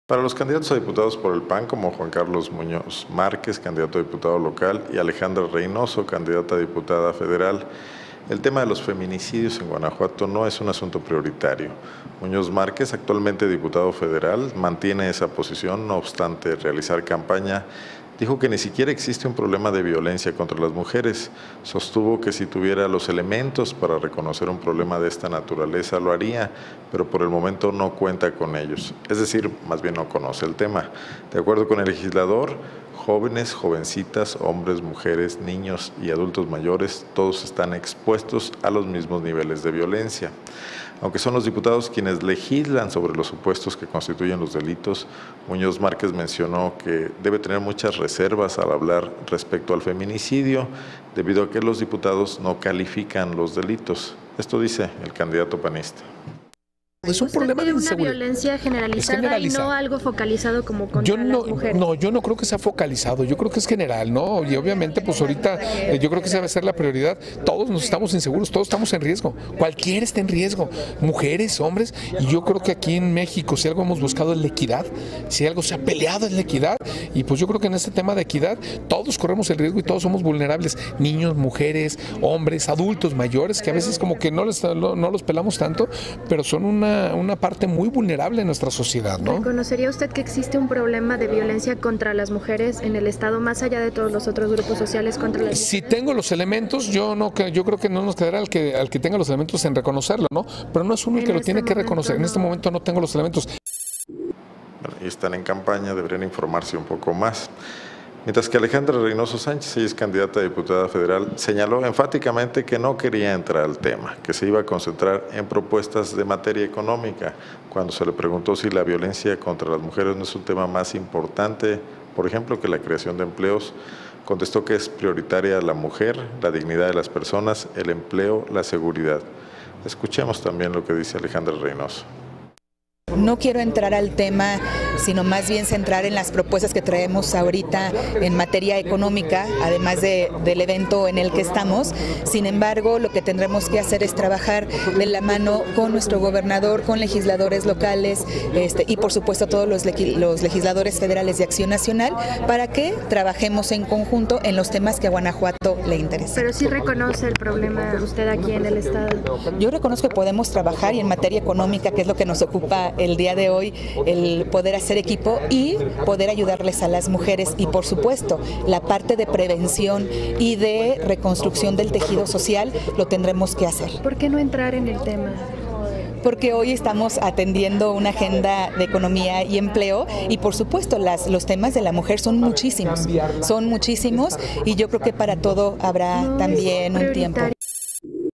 Feminicidio, tema secundario para Juan Carlos Muñoz y Alejandra Reynoso
Mientras que el candidato negó que exista un problema de violencia contra las mujeres, la aspirante mencionó que ésta no es su prioridad